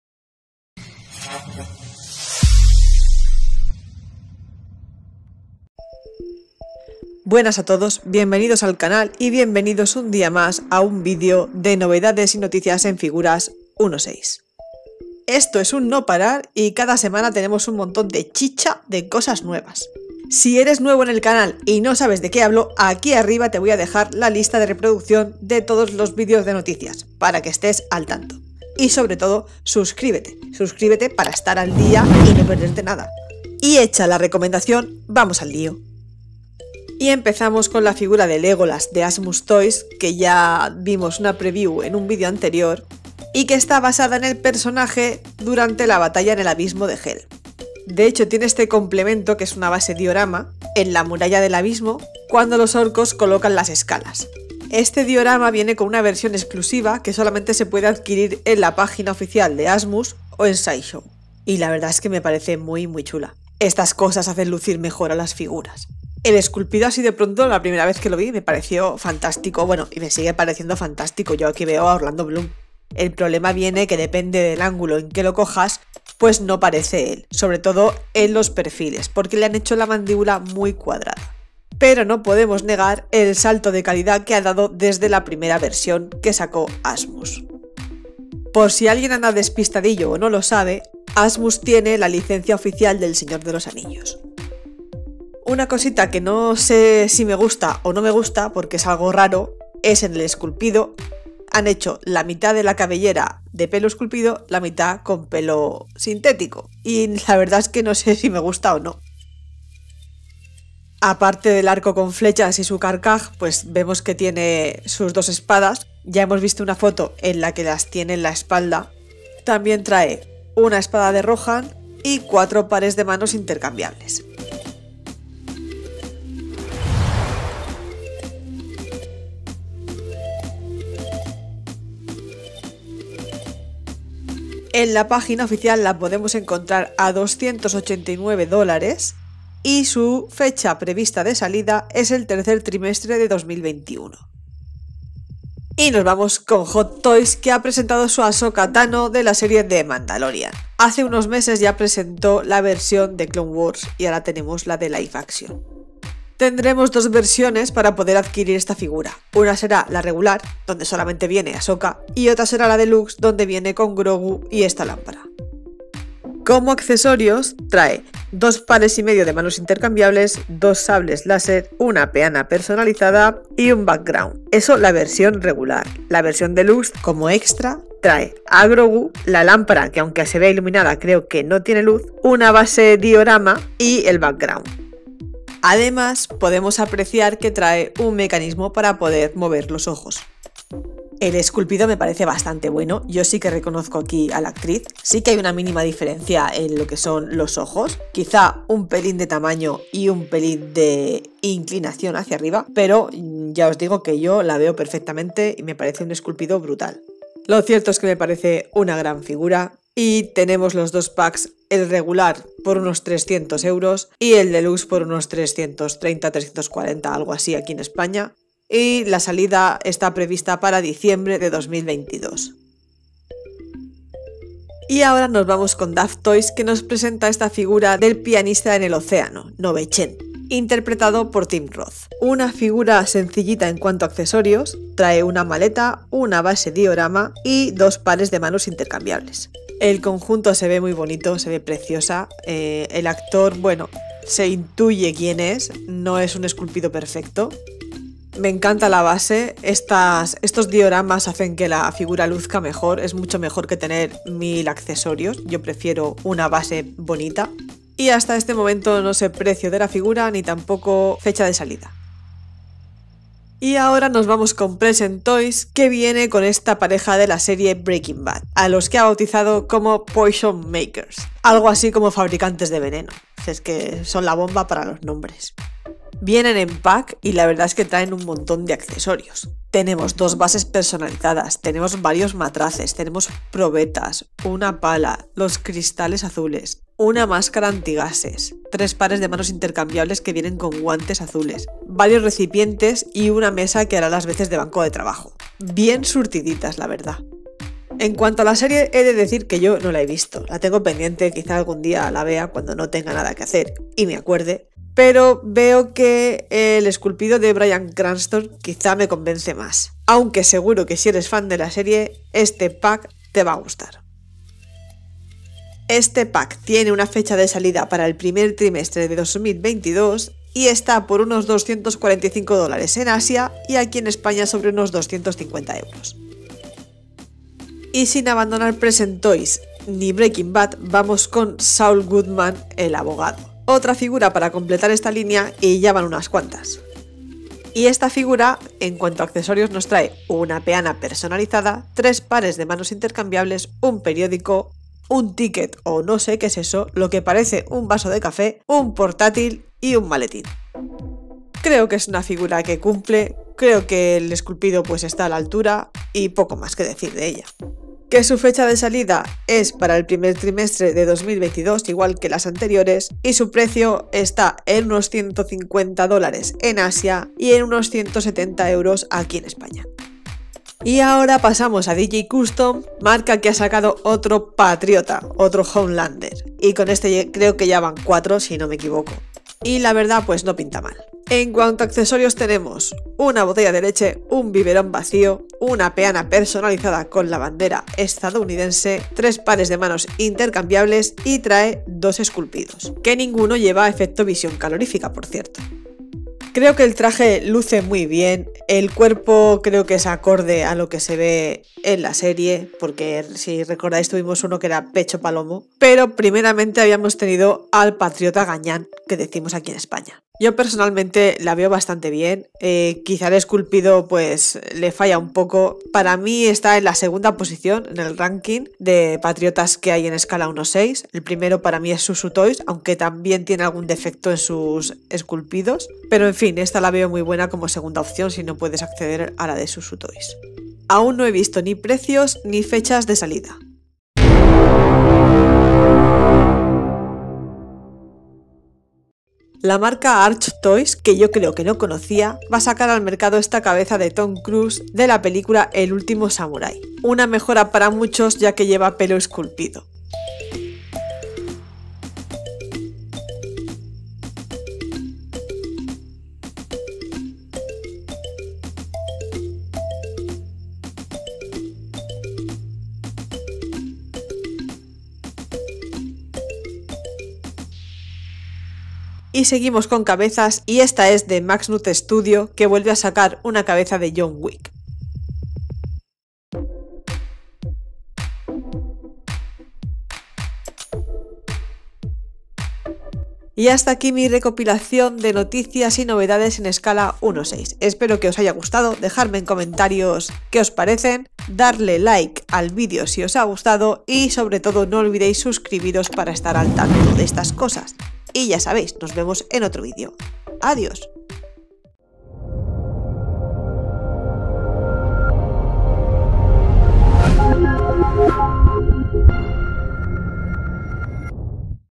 Legolas, Ashoka Tano, Homelander, Breaking Bad, Daftoys, Maxnuy Studio y MÁS!
Asmus Toys nos presenta se versión mejorada de Legolas, en esta ocasión perteneciente a la batalla del abismo de Helm, Hot Toys a Ashoka junto con Grogu de la serie The Mandalorian. A Daftoys con el protagonista de la película La leyenda del pianista en el océano y Present Toys con tres integrantes de la serie de Breaking Bad. Tenemos otro Patriota (Homelander) de la mano de DJ-Customs y un par de cabezas customs!!